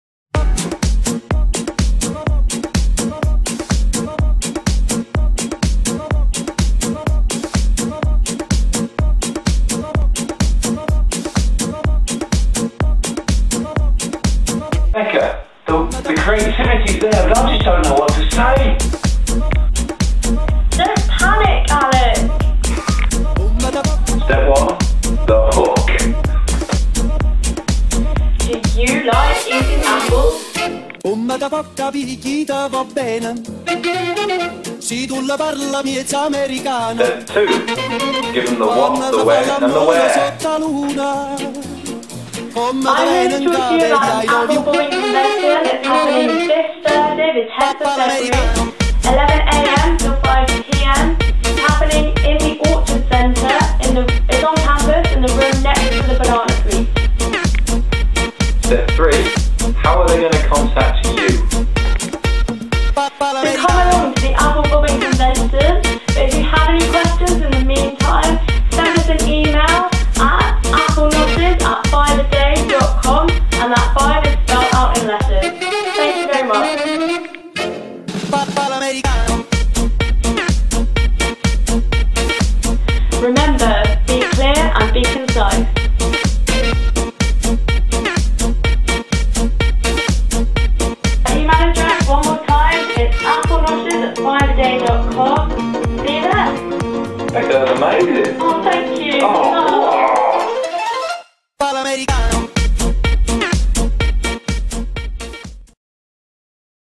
Lecture shout tutorial!
A quick video on how to do a lecture shout for AIESEC